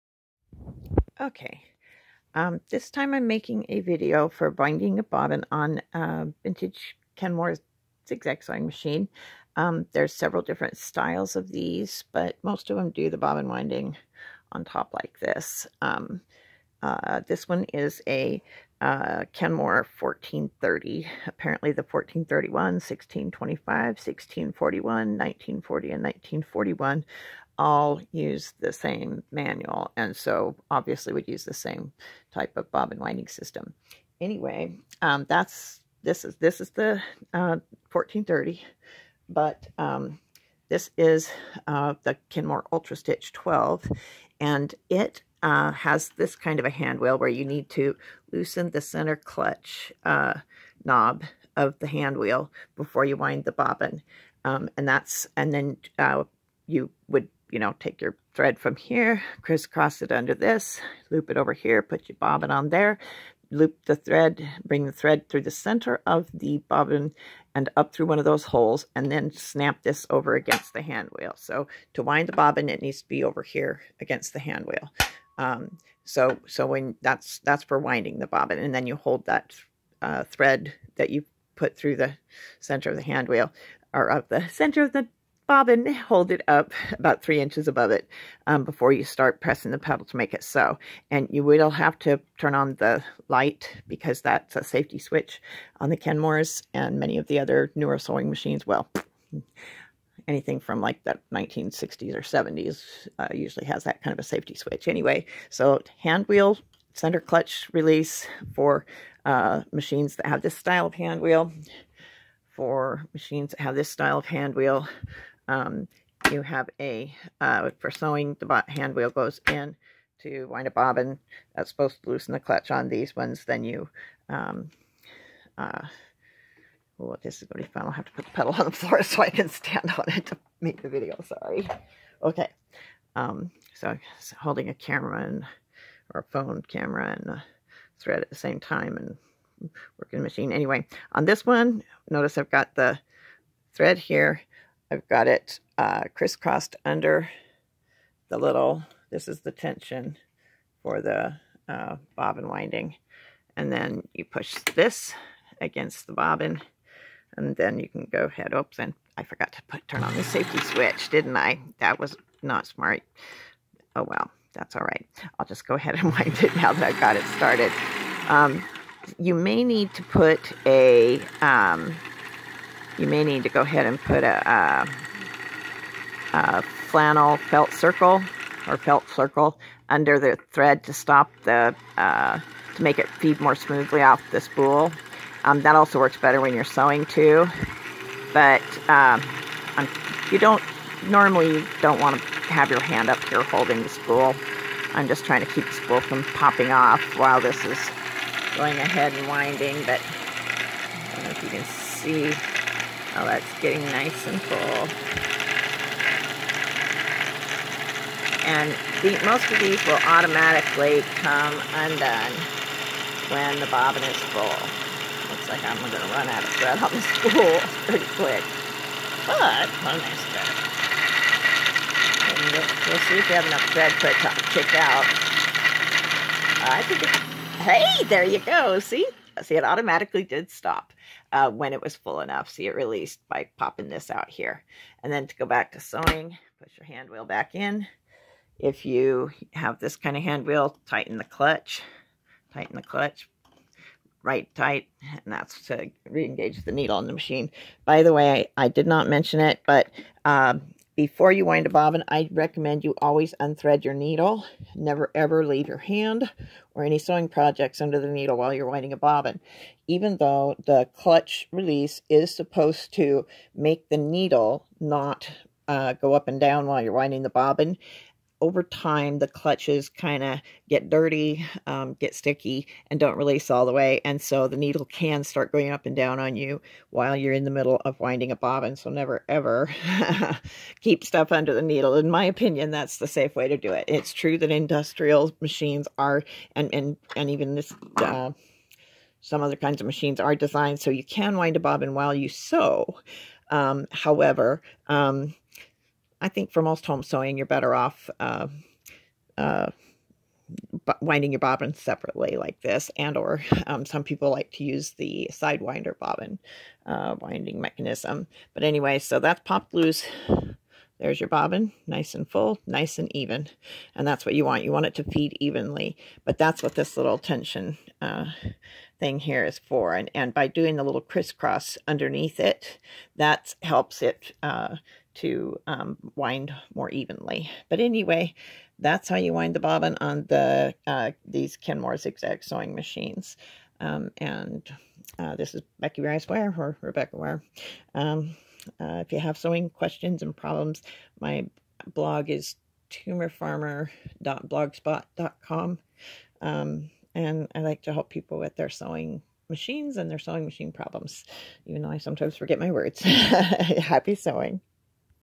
Kenmore sewing machine how to wind a bobbin
Winding a bobbin is an essential skill, for sewing with lockstitch sewing machines.